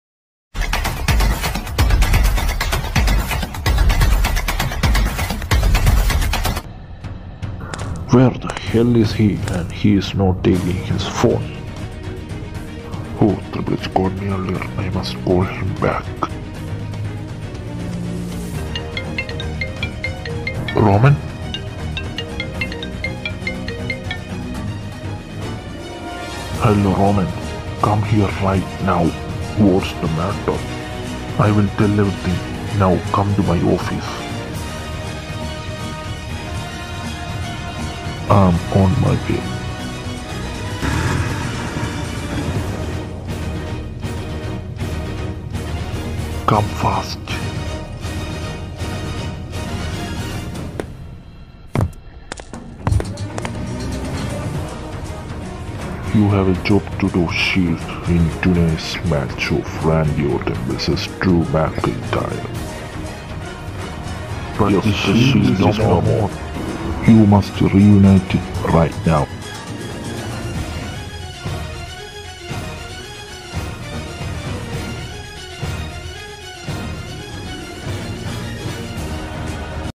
WWE Animated Story Mode - 3 | The Shield Reunion
I am focussing mainly in wwe characters and I had got many characters that I had made . If u want any characters comment .

Like This Video If u Like My Video Guys
Help Me To get 1k subs and I am giving many giveaways after that. See U in the next video friends
Mrz animation